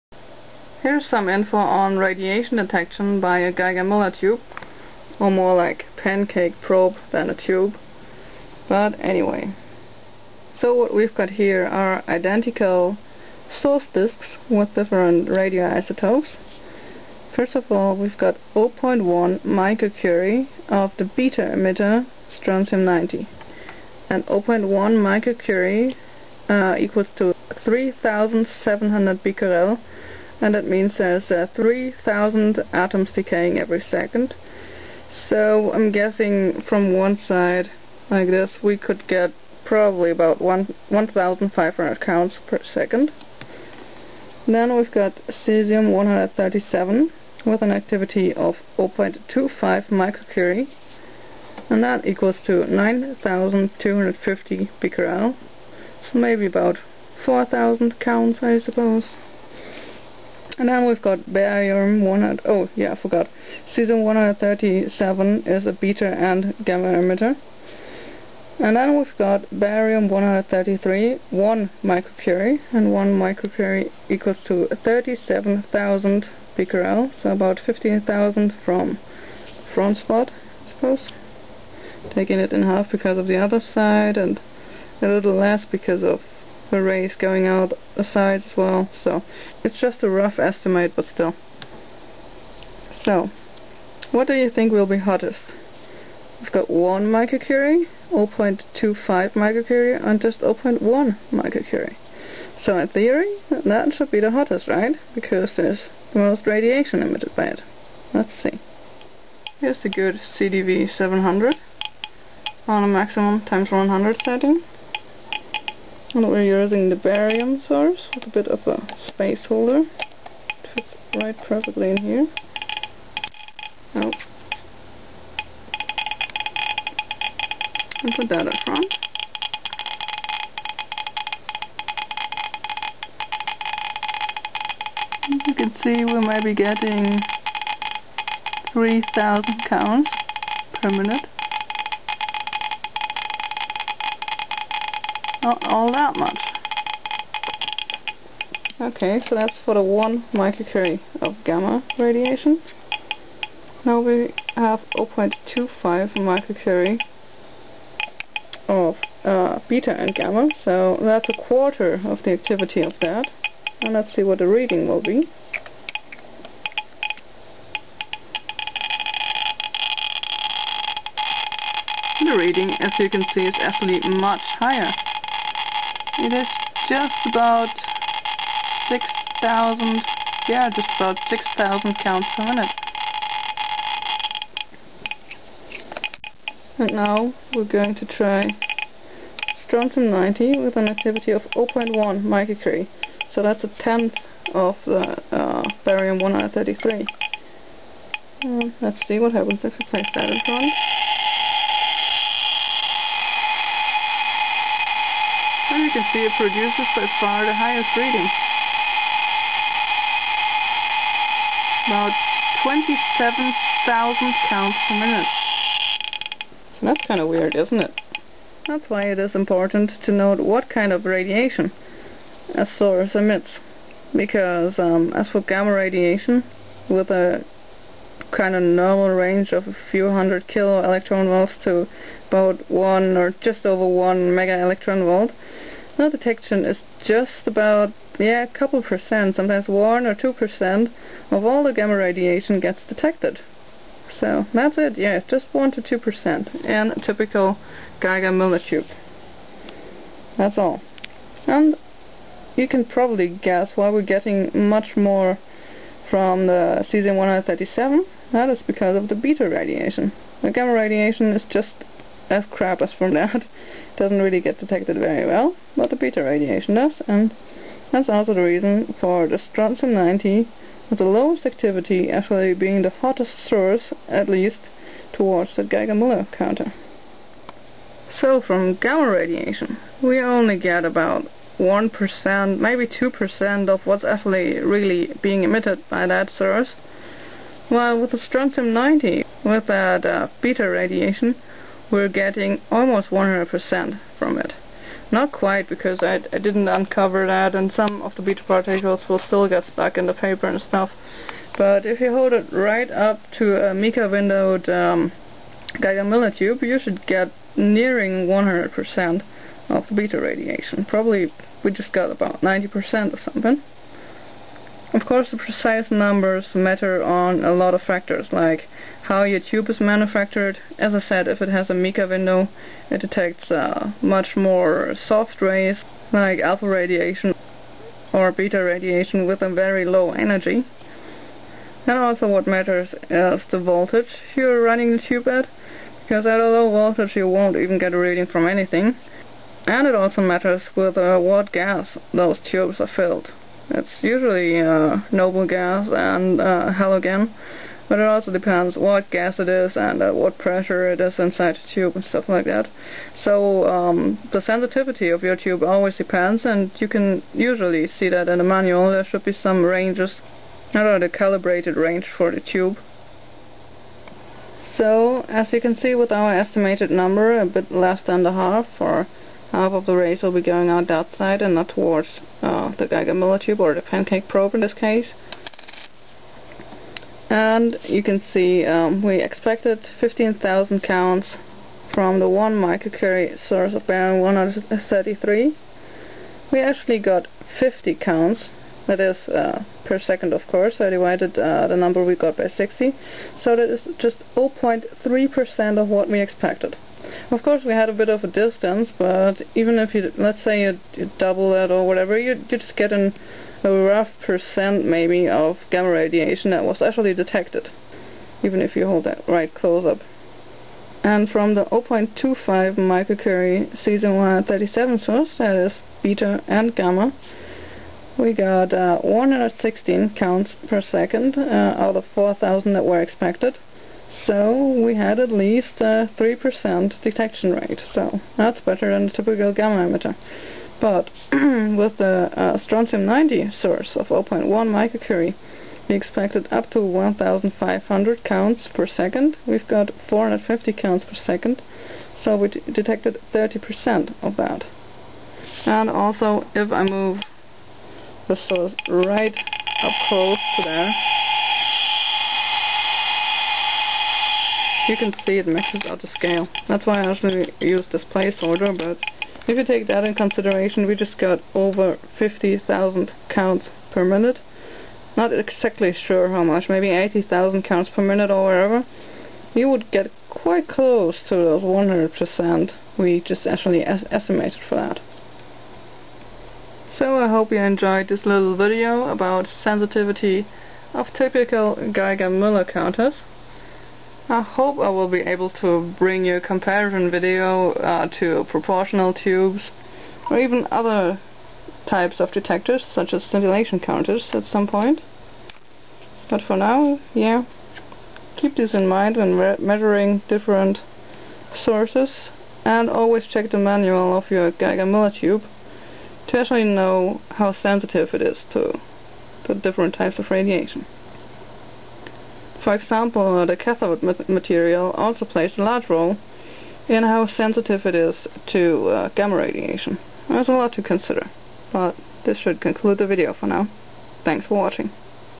radiation detection with a geiger mueller tube - sensitivity / efficiency
everybody knows that gamma radiation is highly penetrating; it just moves through basically everything without being stopped (without interacting with the matter).
beta radiation, however, is much more "densely" ionizing and is stopped much easier - now, what does that mean in terms of detection for these types of radiation?
this video holds the answer. :)

as a side note, alpha radiation has a very good detection rate on a mica-windowed tube as well, maybe 80% (some of the alphas still get "stuck" in the window material) - but i didnt demonstrate this as i dont have an equal alpha source disk. also, alpha radiation is usually of no concern due to its very short range in air (1-2 inches).